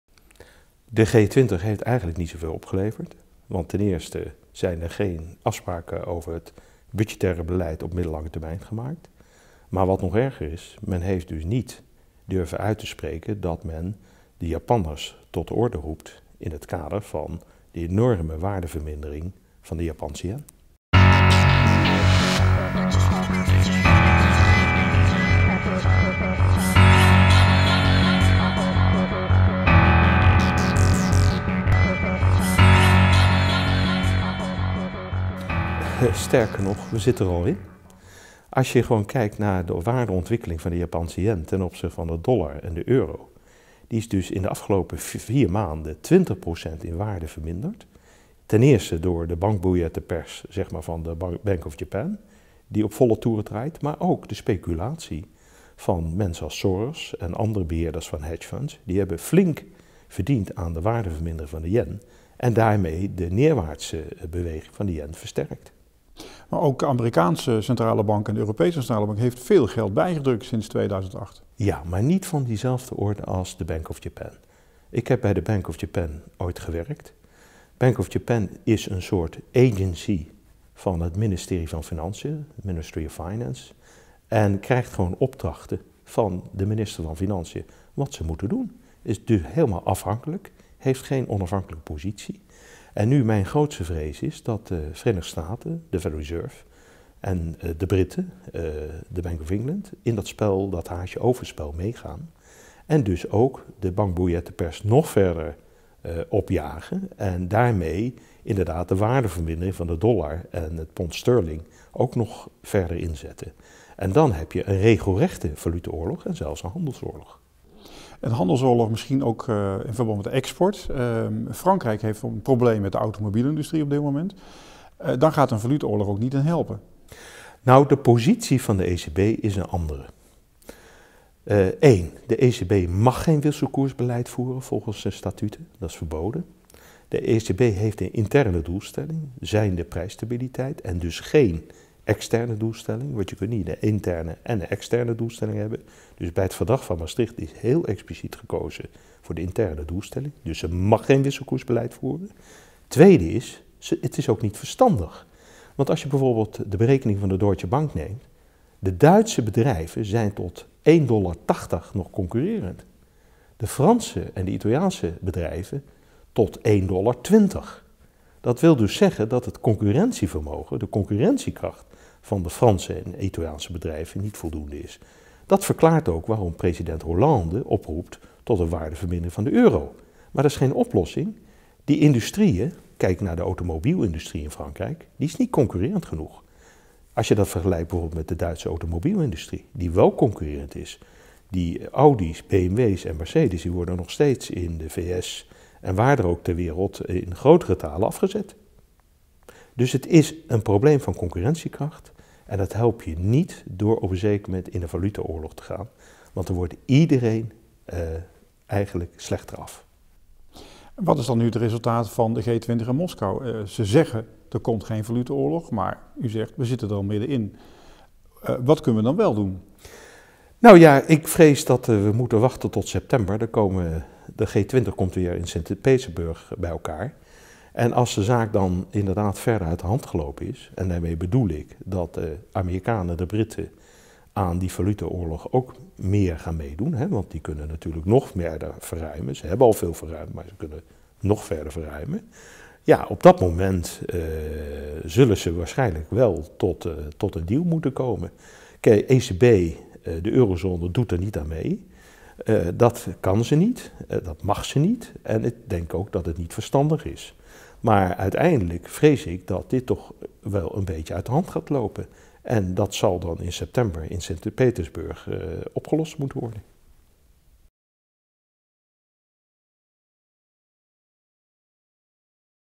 Econtrack - Sylvester Eijffinger over de valutaoorlog
We zitten al midden in een valutaoorlog stelt Sylvester Eijffinger, hoogleraar financiële economie Tilburg University. De Japanse Yen is de afgelopen 4 maanden met 20% in waarde verminderd door enerzijds het beleid van de Bank of Japan en anderzijds door speculaties.